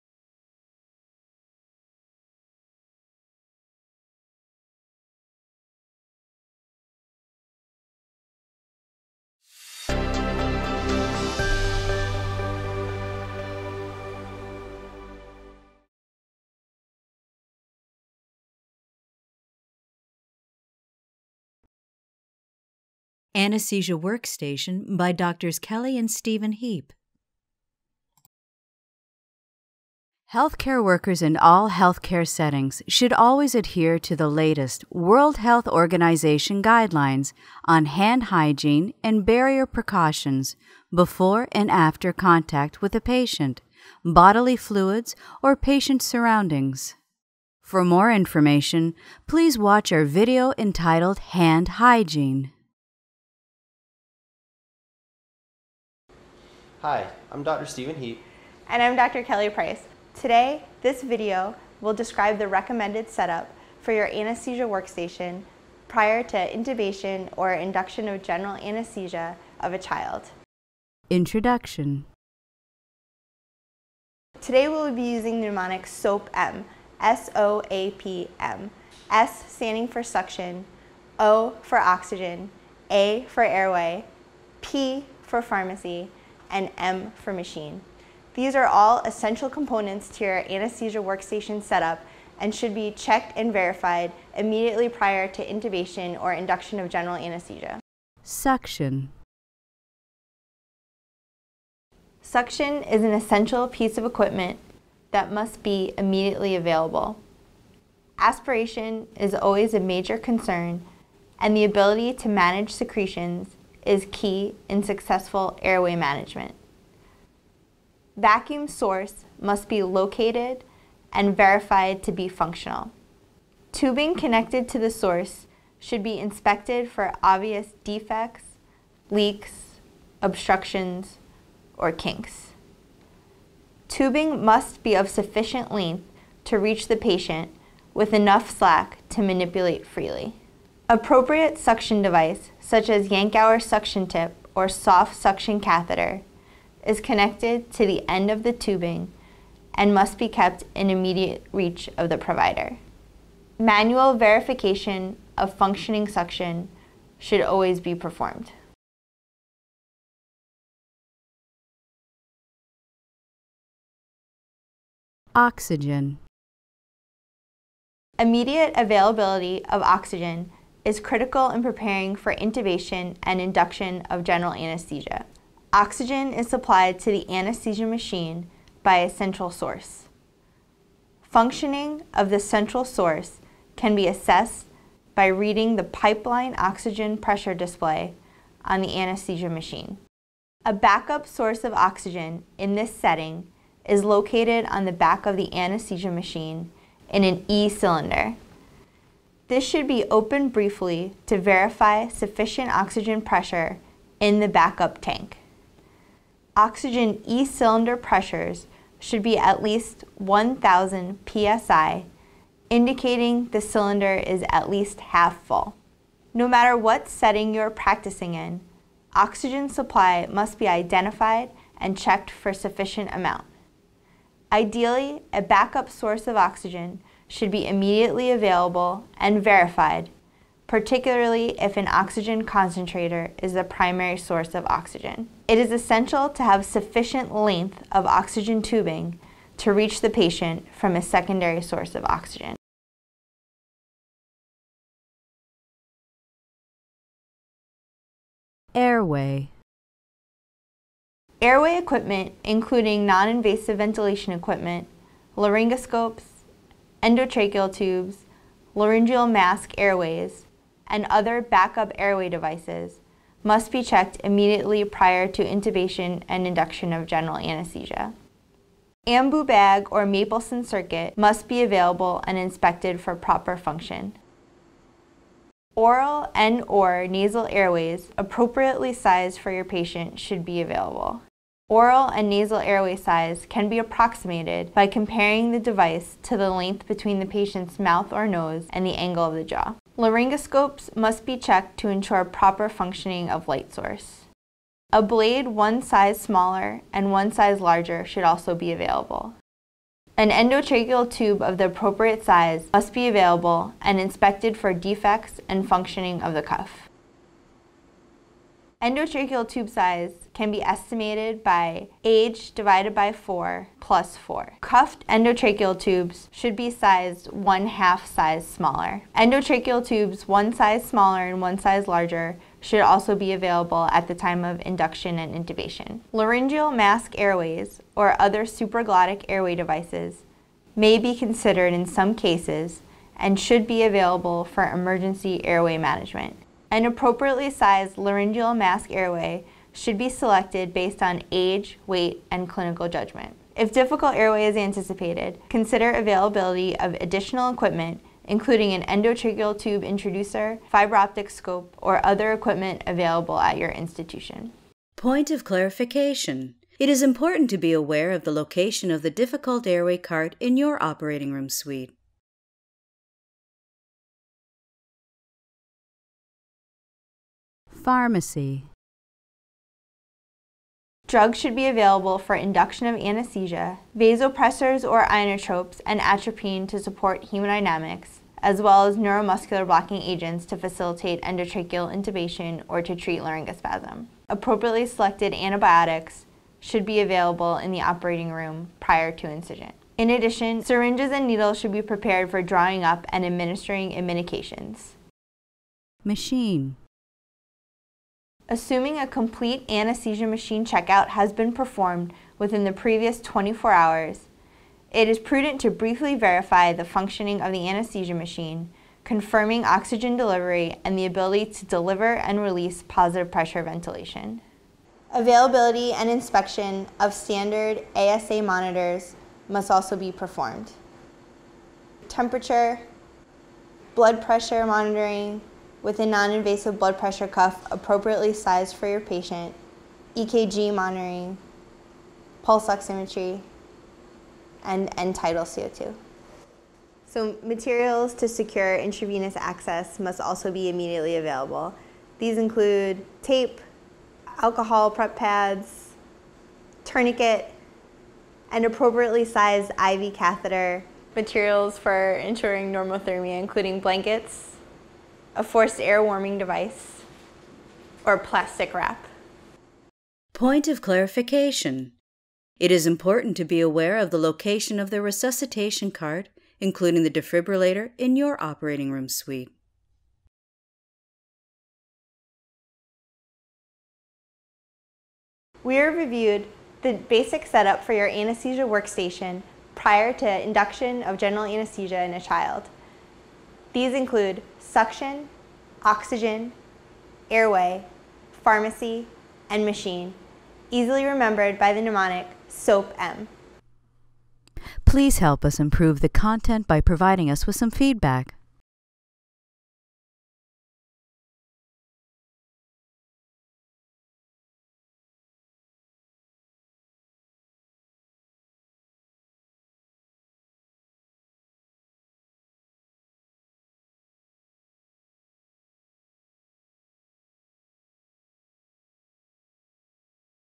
Anesthesia Workstation by K. Heape | OPENPediatrics
In this video, Drs. Kelly and Stephen Heape demonstrate recommended organization and preparation of the anesthesia workstation prior to intubation or induction of general anesthesia of a child.
1:13 Chapter 1: Introduction
1:37 Chapter 2: Suction
2:44 Chapter 3: Oxygen
4:08 Chapter 4: Airway
6:07 Chapter 5: Pharmacy
6:37 Chapter 6: Machine